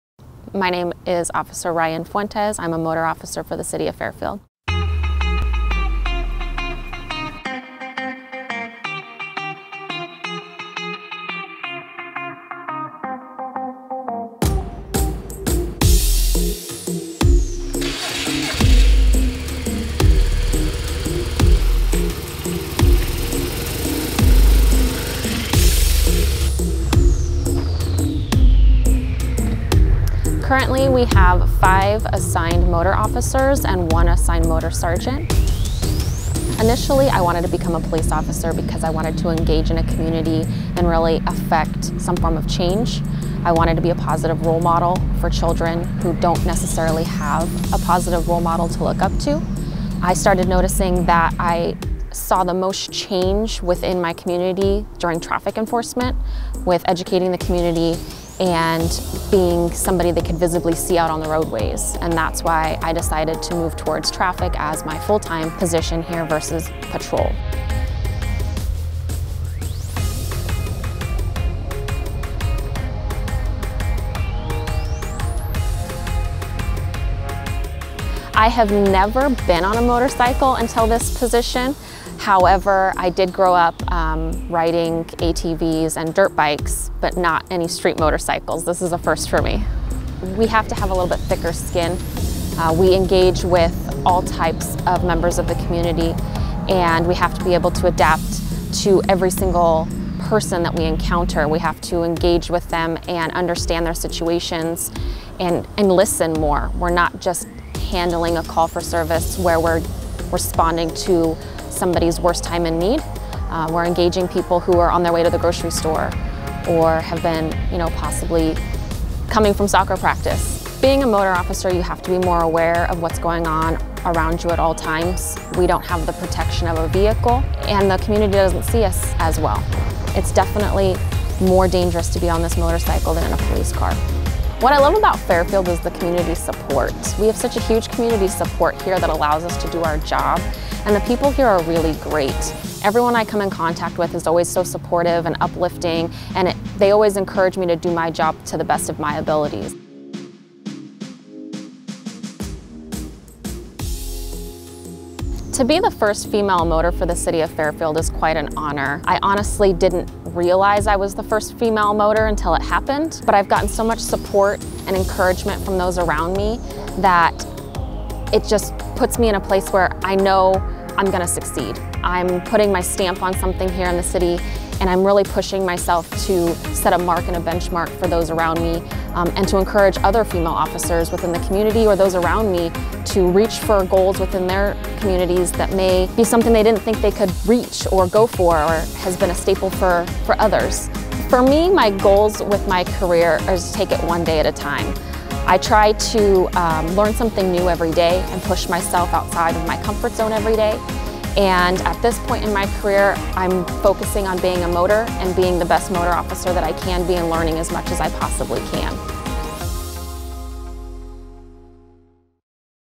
Meet Officer Fuentes
In law enforcement, motor officers are distinguished as the top 2% of this field. Female motor officers comprise the top 0.2% of law enforcement. Ryann Fuentes is Fairfield's first female motor officer